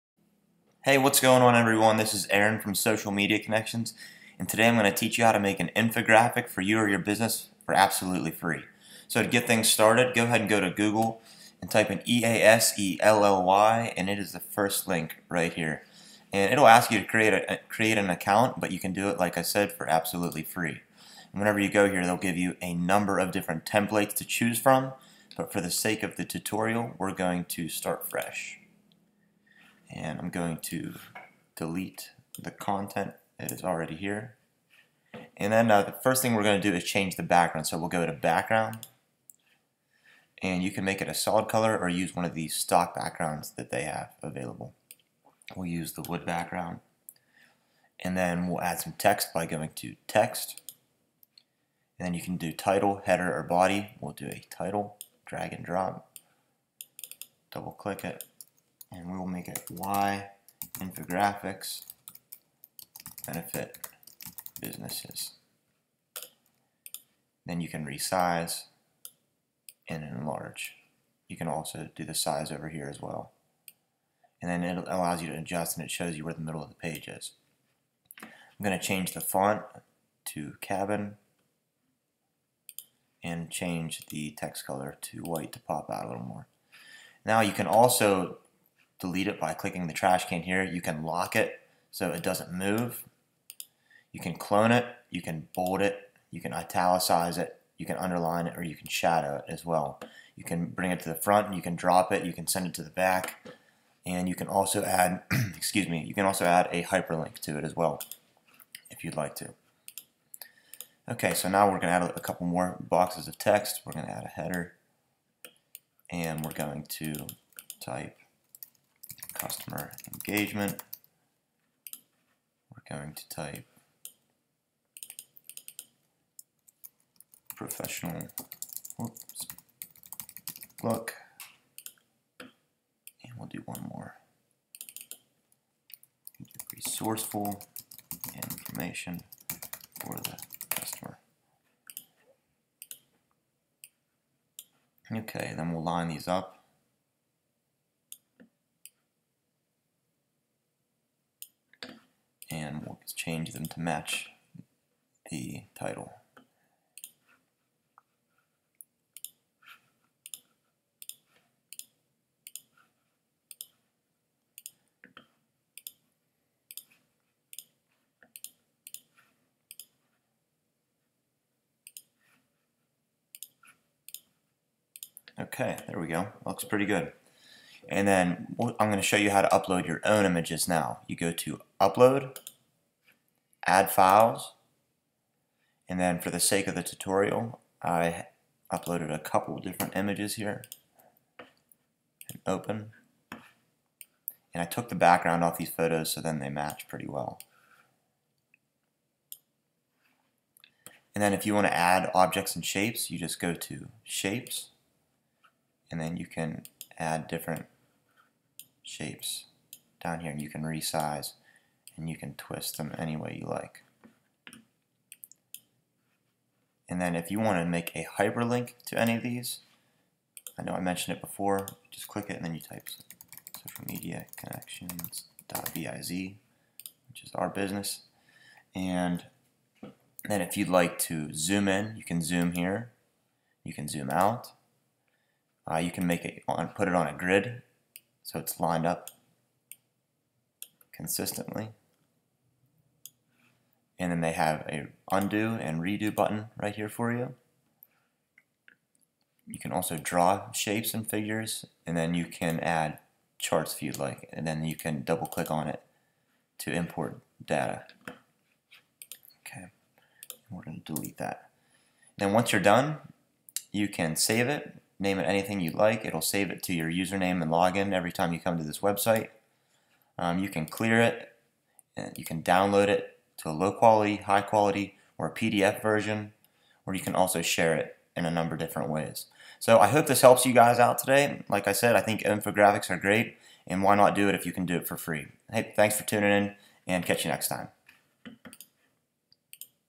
Infographic Tutorial - How To Create an Infographic For FREE
With all of the information being distributed by businesses online, it's can be tough to grab your customers' attention.  Thanks to easel.ly, you can now grab your their attention by creating professional, FREE infographics.